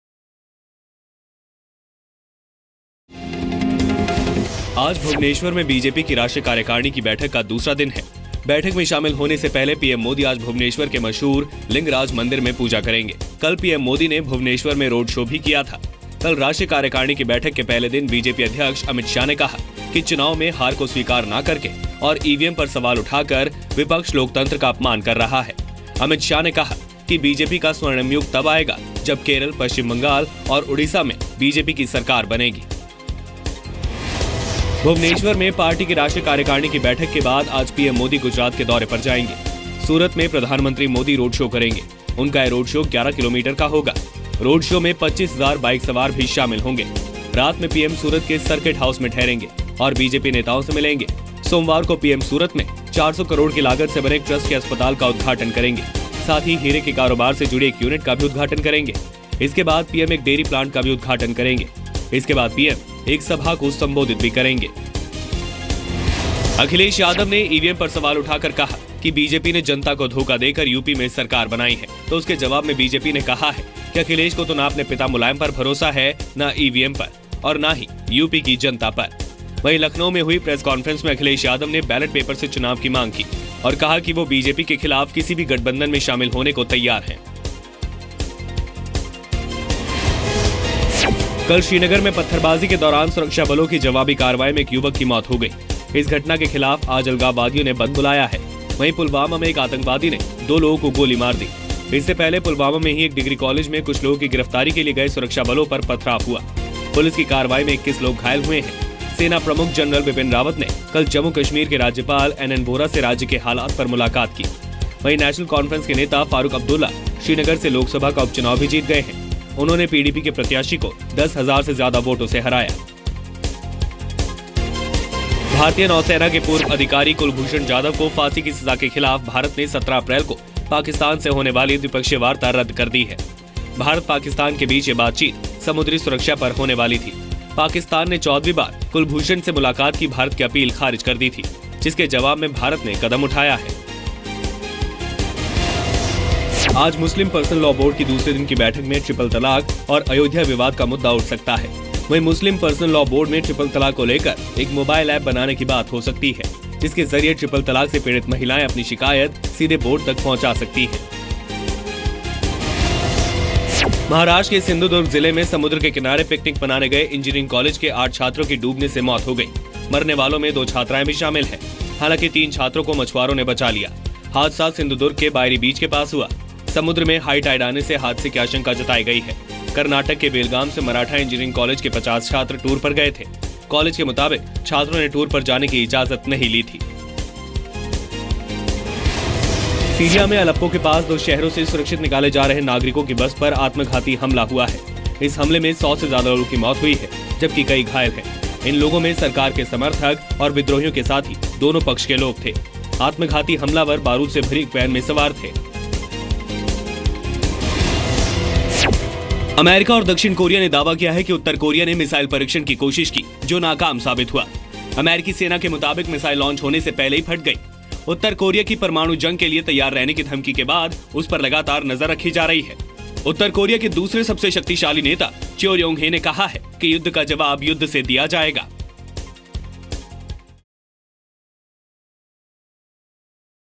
Akhilesh Yadav raised questions on the authenticity of EVM machines in UP assembly elections
Akhilesh Yadav raised questions on the authenticity of EVM machines in UP assembly elections. Watch this special segment and get to know more here.